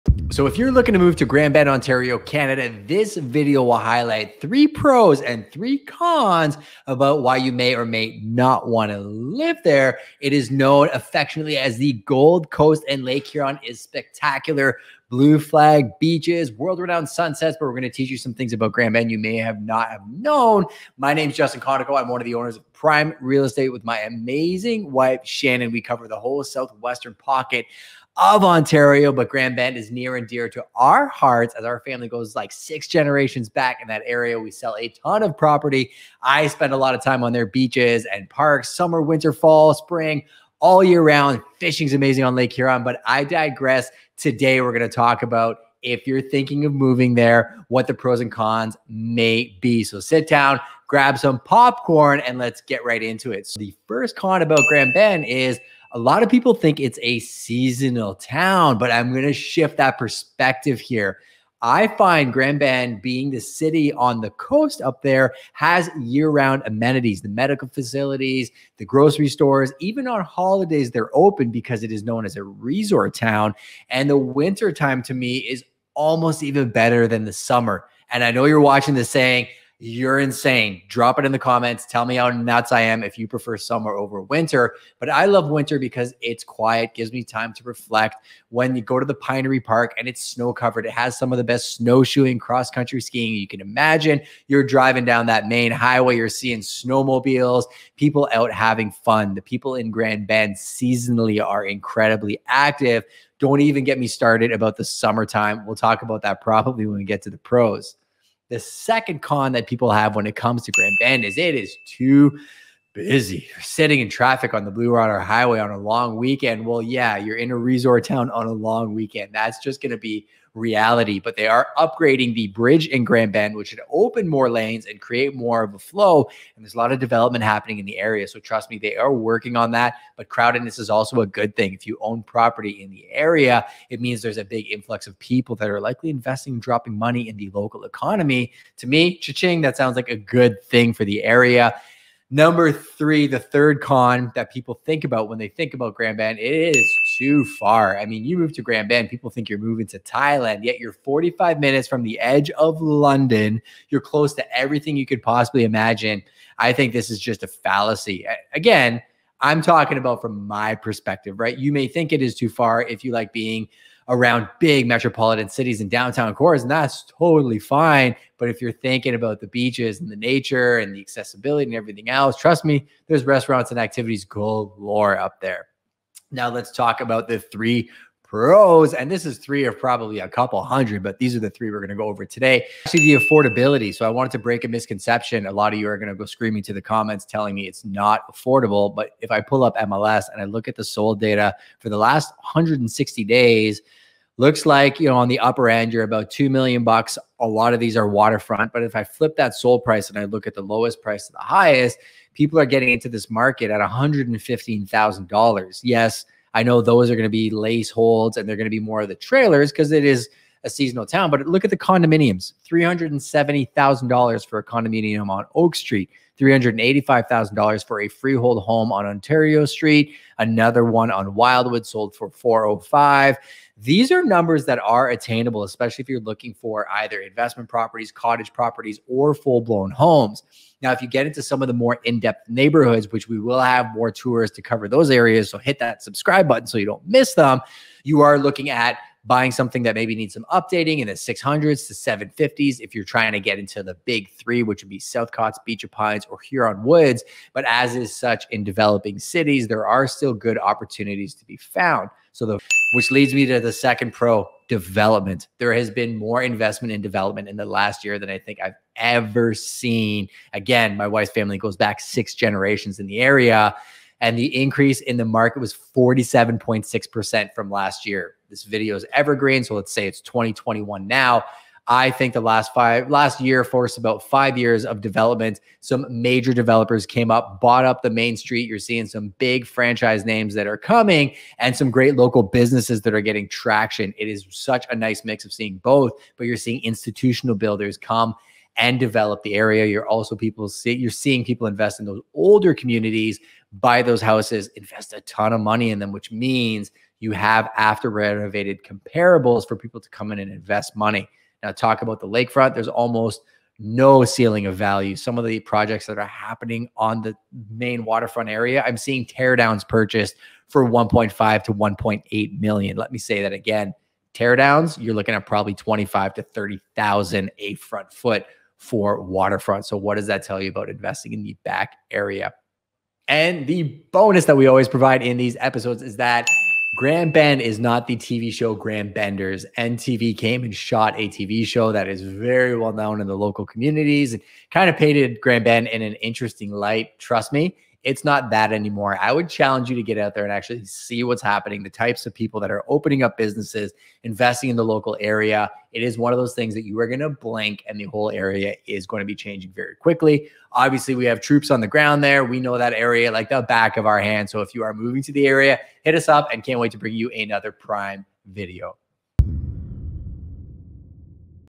Pros & Cons of Living in Grand Bend Ontario
Today we talk about the 3 cons and 3 pros of living in Grand Bend, Ontario, Canada. This is a well-known beach town off the water of Lake Huron, in Lambton County.

Like all small towns though, it does come with some ups and downs that you should take into consideration before deciding on making the big move, although Grand Bend has plenty to offer you as a future resident of the community.

Justin Konikow, the owner of Prime Real Estate based out of London sits down and gives you the 3 pros and 3 cons of living in the heart of Grand Bend, Ontario.

If you're thinking about relocating to Grand Bend in upper Lambton County, this video is for you! Sit down, grab some popcorn and this video can help you decide if this is the right city for you.

0:00 - Introduction
0:53 - Con: It's Only A Seasonal Town
1:50 - Con: It's Too Busy
2:26 - Con: It's Too Far
3:02 - Pro: The Affordability
4:12 - Pro: New & Continuous Development
5:26 - Pro (Bonus): It's Not Grand Benders TV Show